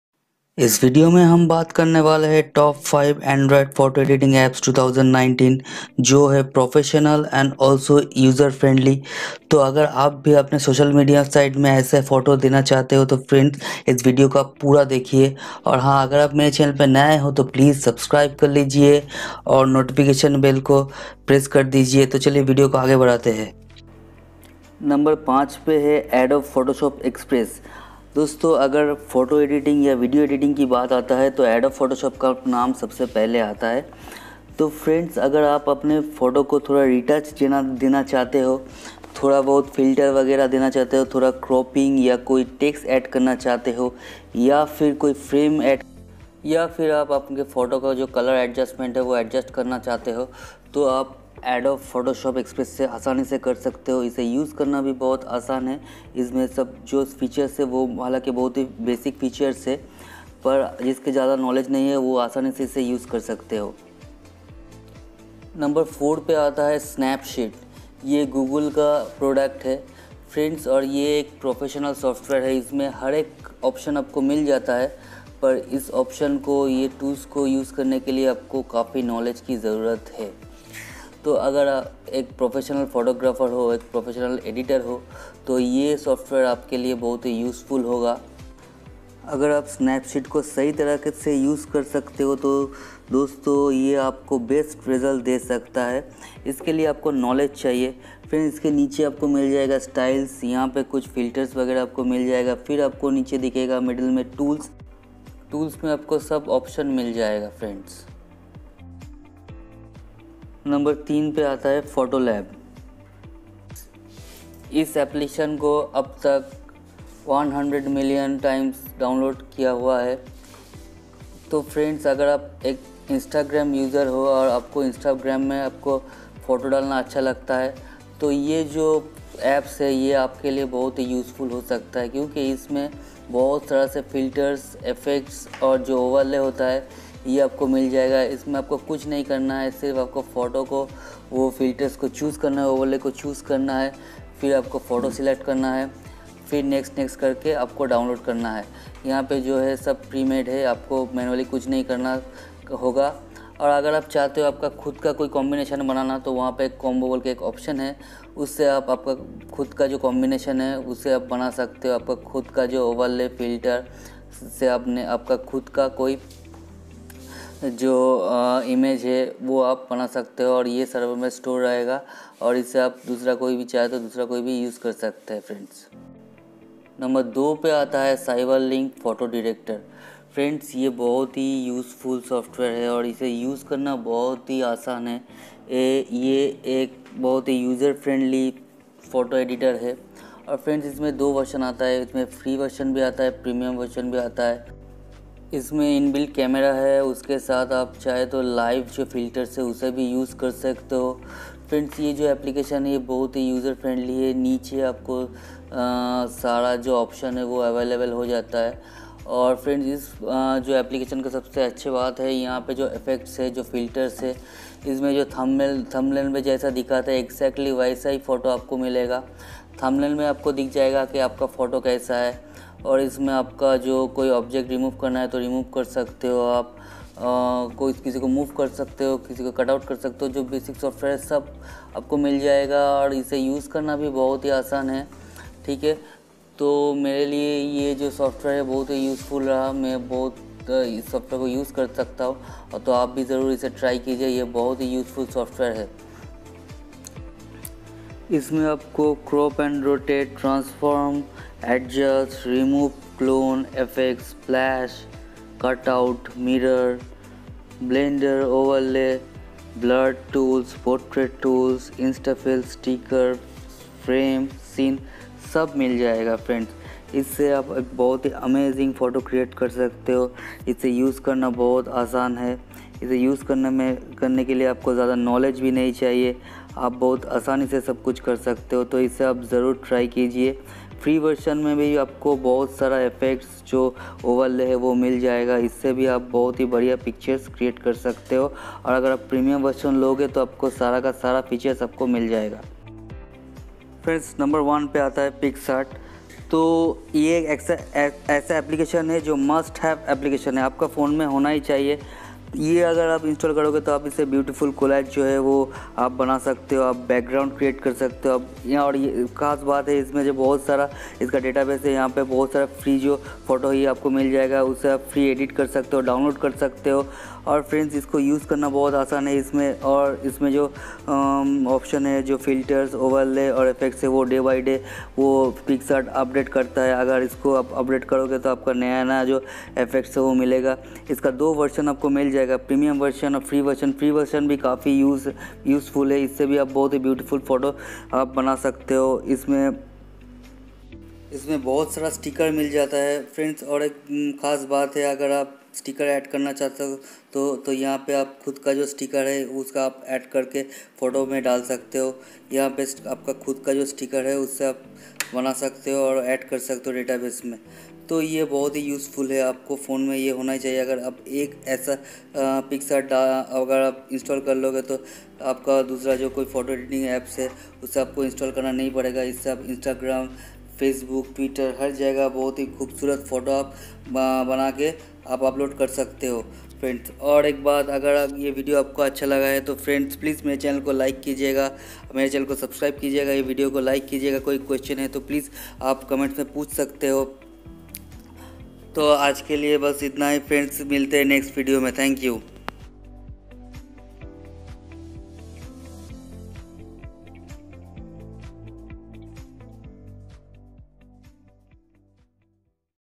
🔥Top 5 Photo Editing Apps For 2019 🔥
Hello friends || We made a list of Top 5 photo editing apps for 2019 . Please watch till the end to understand features of each app. I hope you guys enjoys this video